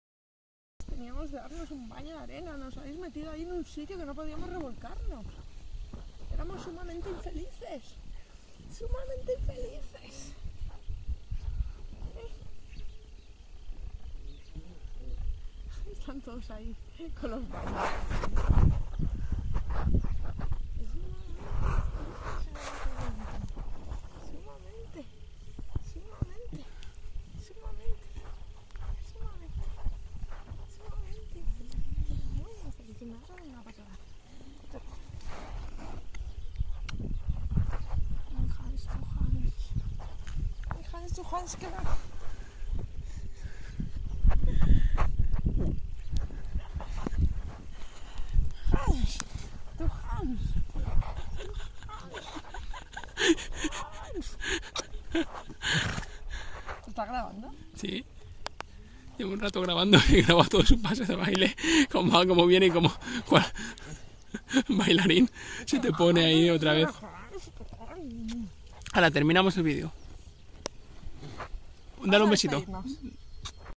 Hansel, Laura, caricias y un baño de arena
Hansel es un burro que rescatamos de una situación de extremo maltrato. Tras ser decomisados, él, junto a su padre y su madre, vinieron al Santuario y desde entonces son muy felices ¡como puedes ver en este vídeo!
Hansel disfruta de las caricias de Laura